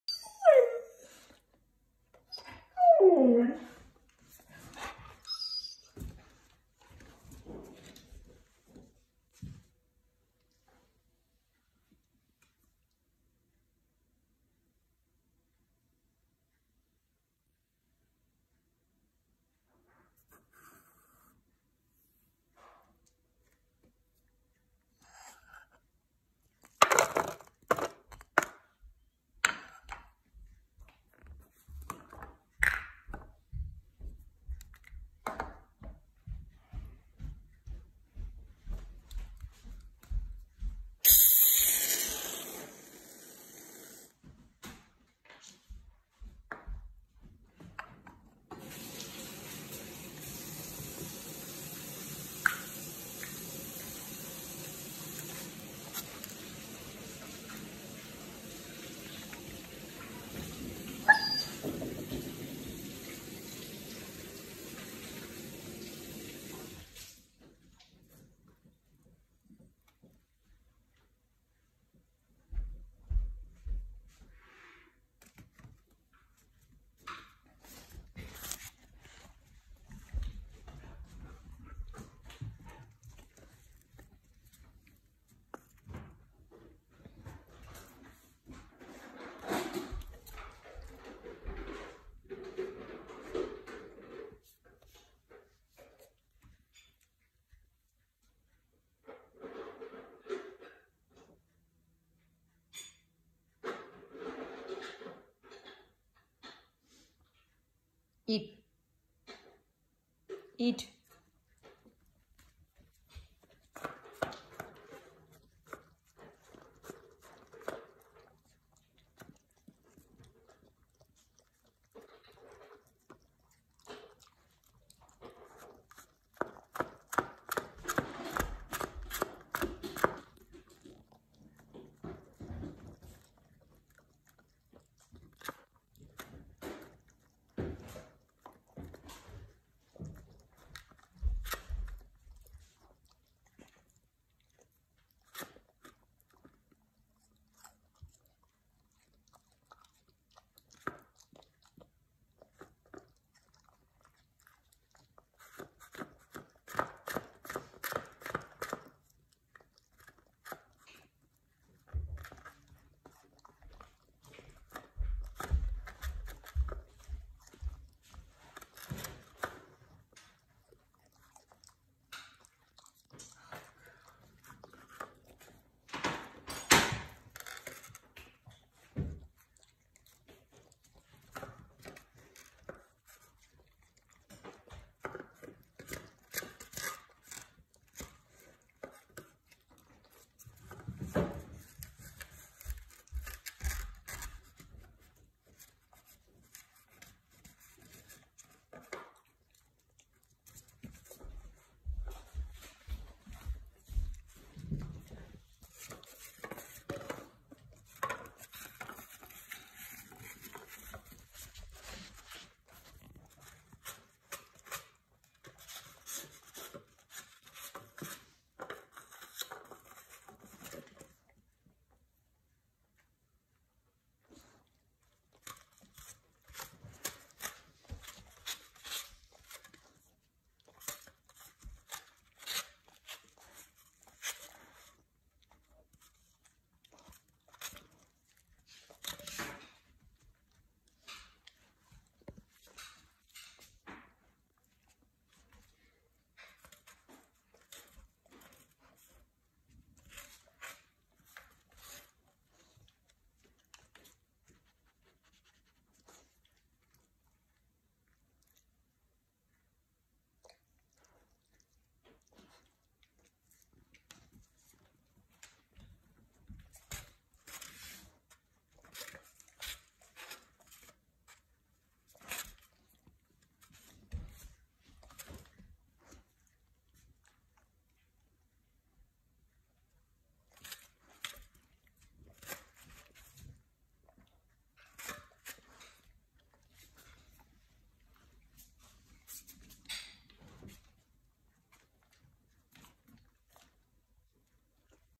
Husky ask for his Favorite Breakfast!
Hi y'all,
Here is the video of Bentley asking for his favorite breakfast.
Are you curious to know what his favorite Breakfast is?
Checkout the video for his favorite Breakfast and how much he enjoys eating it!!